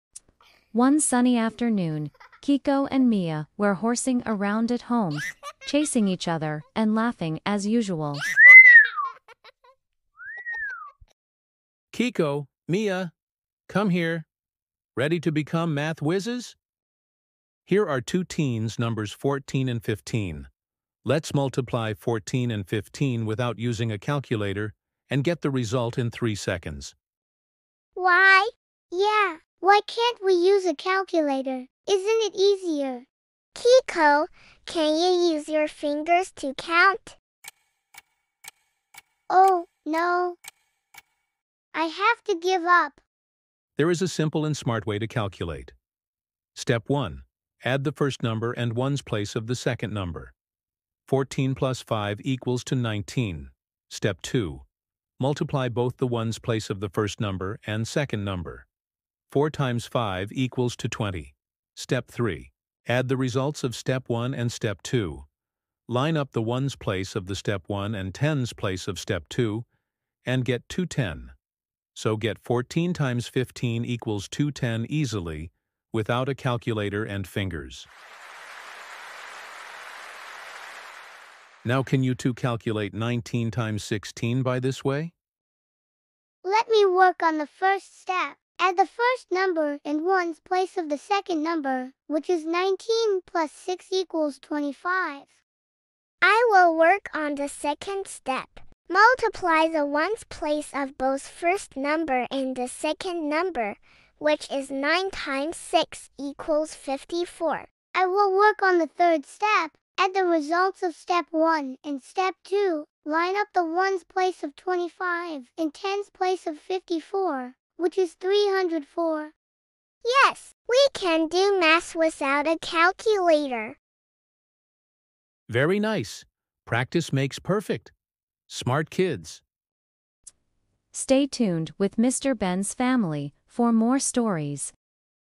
Kiko and Mia's smart calculation | Kids Bedtime story, math, fun, learn
Mr. Ben taught Kiko and Mia a smarter way to do calculation.